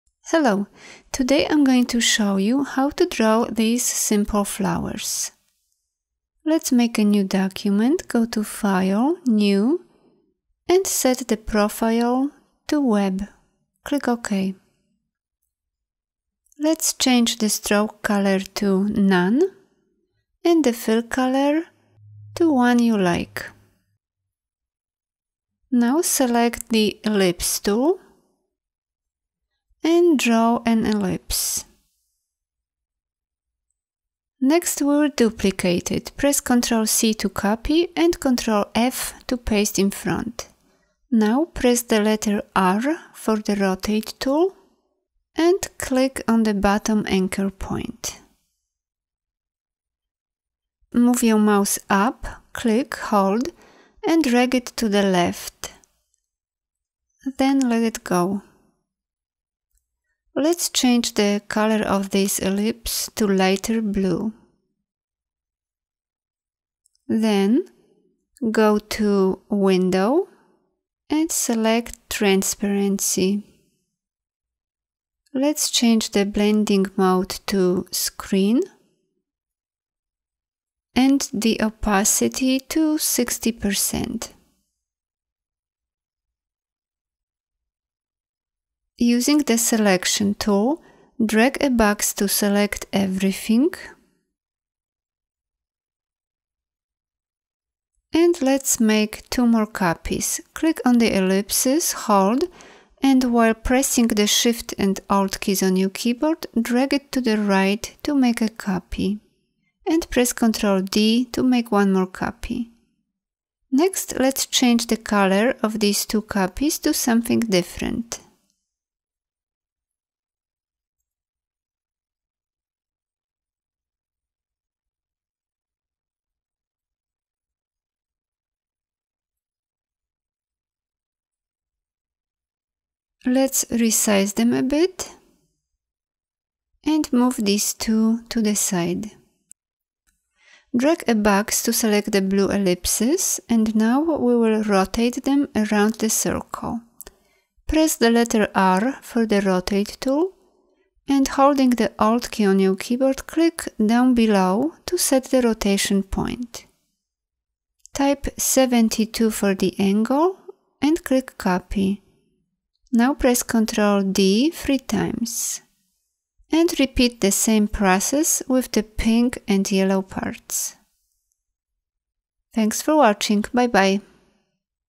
How to Draw Flowers in Adobe Illustrator
Very easy and fast way to draw flowers using the ellipse and rotate tools. More tutorials available here: (click on SHOW MORE)
TUTORIALS